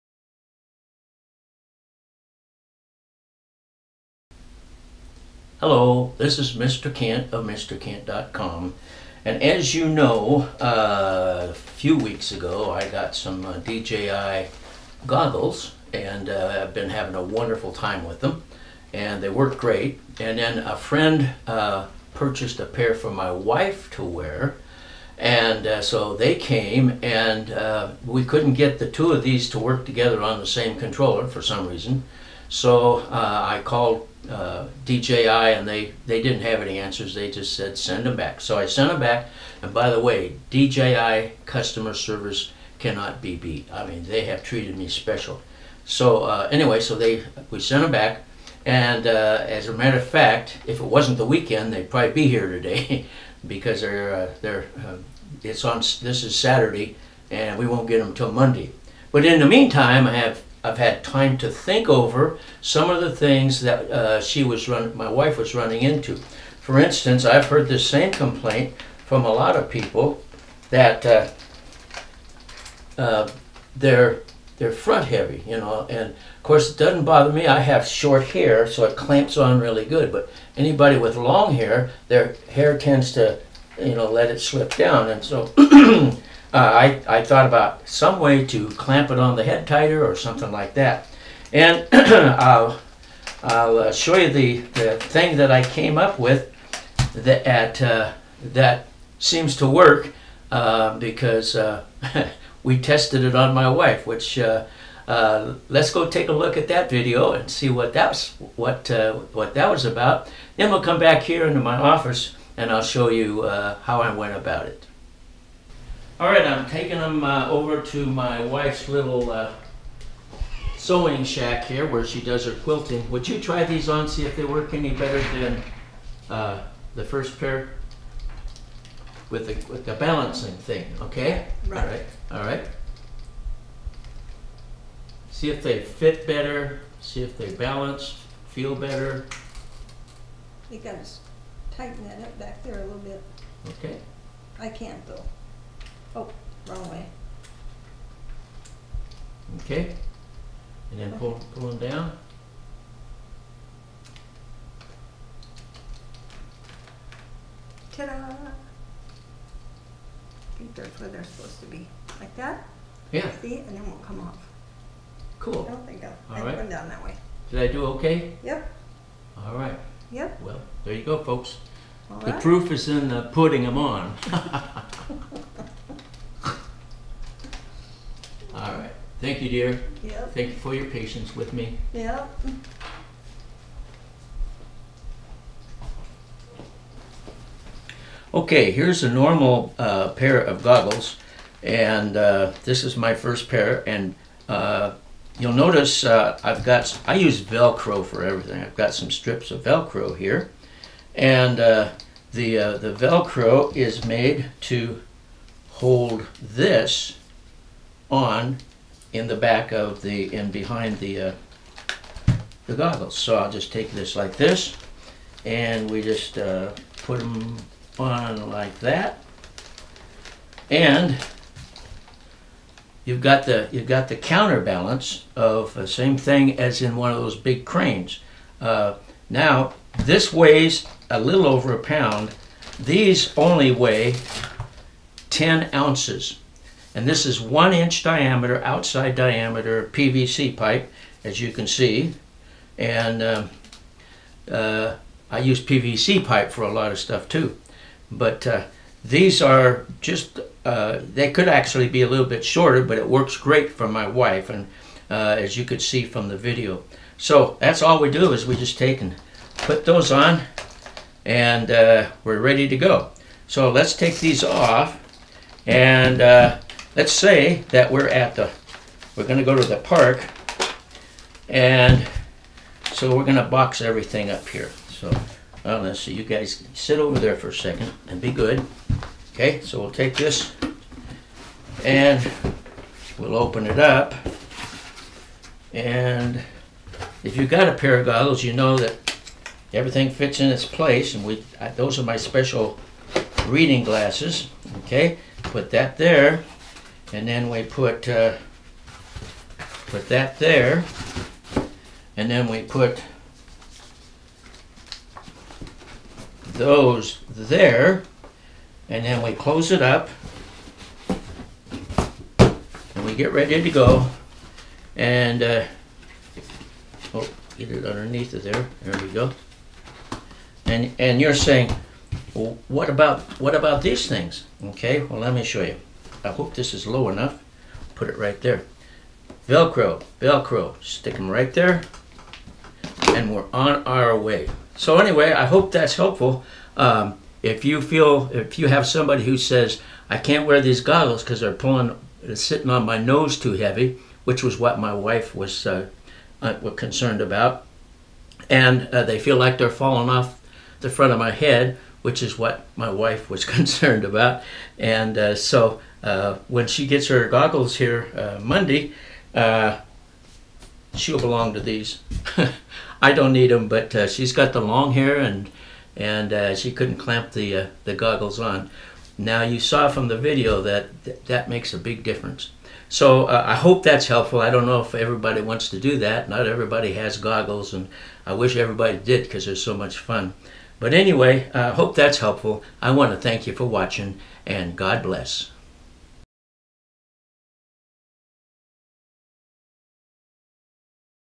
DJI GOGGLES BALANCED ON YOUR HEAD
If you have been following my DJI Goggles videos you know I had to send the second pair back to DJI because we had no success at connecting to pair of goggles to the controller.

FYI - DJI CUSTOMER SERVICE IS TOPS!

Other issues we had with my wife's goggles was that she had difficulty keeping them from sliding down in the front, and they didn't seem to fit right. So, while we were waiting for the exchange pair to arrive I thought about those issues and came up with I demonstrate in the video.

I used PVC pipe which anyone can obtain for practically nothing - and - Velcro tape. In the video you may notice that I didn't put enough Velcro on the balancer but that is an easy fix.

If you have questions please don't hesitate to ask. Enjoy and God bless.

Crane picture courtesy of Pixabay.com